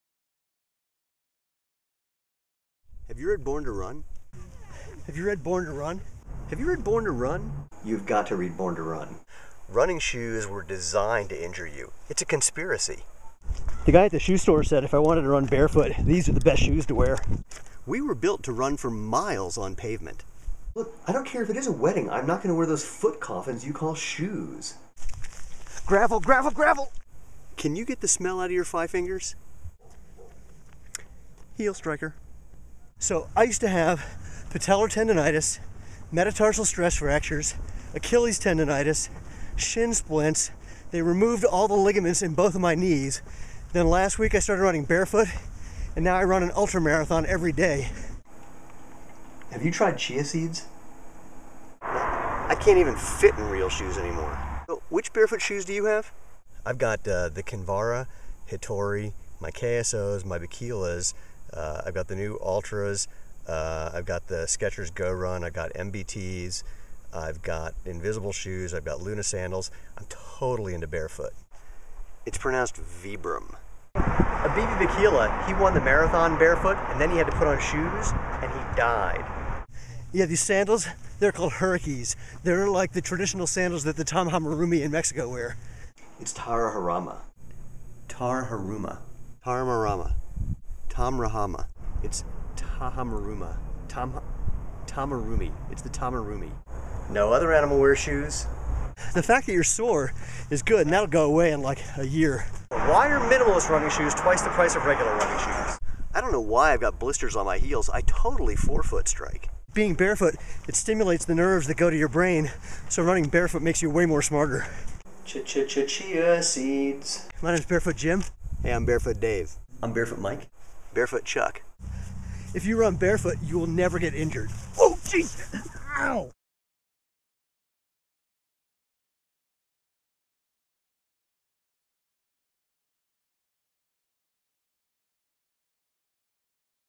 Sh*t Barefoot Runners Say from Xero Shoes
- The latest in the "Sh*t ___ Say" series. Barefoot runners, how many of these have YOU said? ;-) (oh, and have you read Born To Run?)